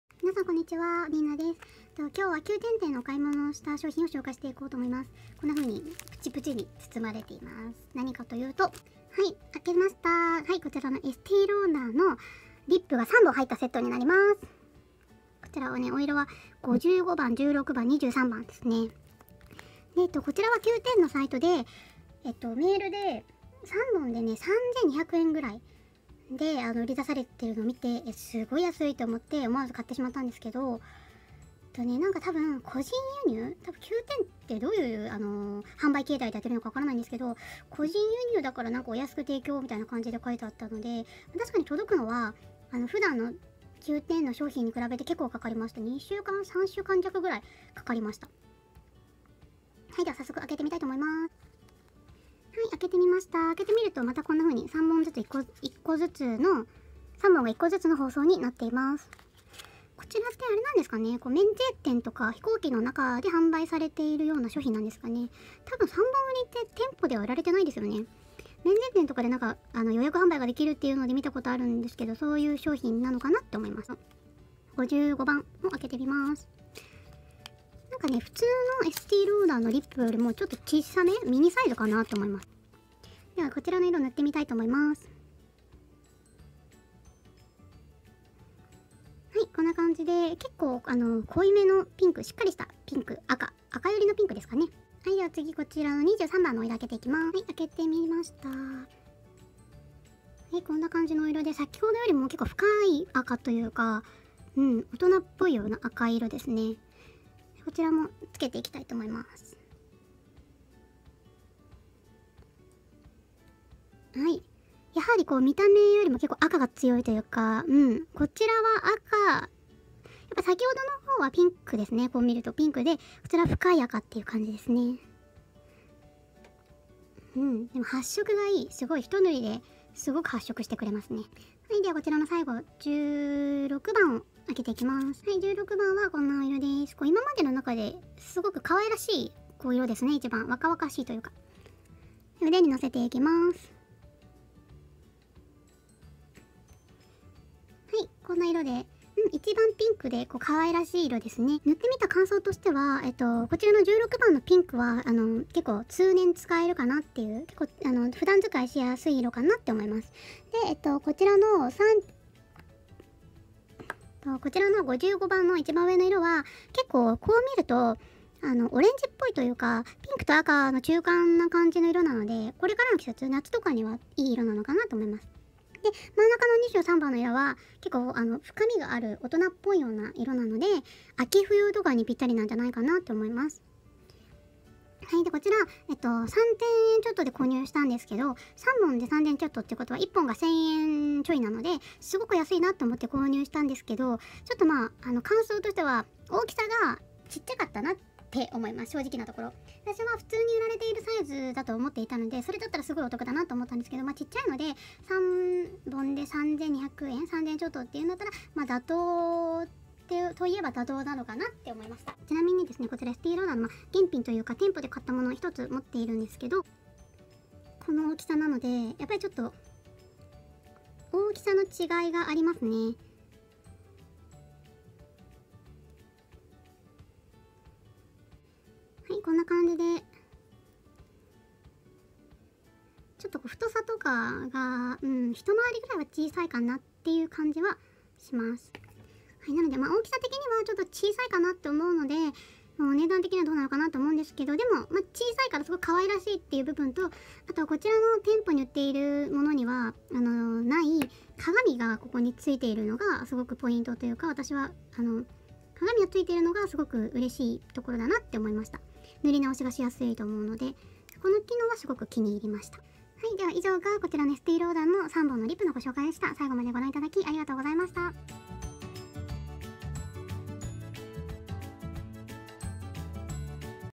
【購入品】【Qoo10】エスティーローダーリップ３本セット
Qoo10で3290円で購入しました(^^)
飛行機内や免税店で購入できるミニリップセットだと思います＼(^^)／色味が気になっていた方のお役に立てれば嬉しいです！